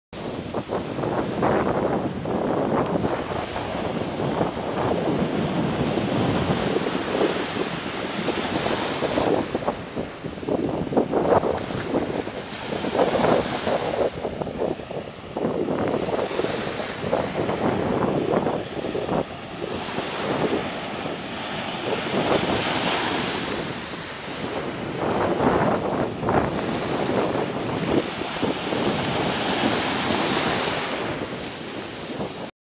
Sea Lion in the Surf
Bathing sea lion. Gardner Bay, Espanola, Galapagos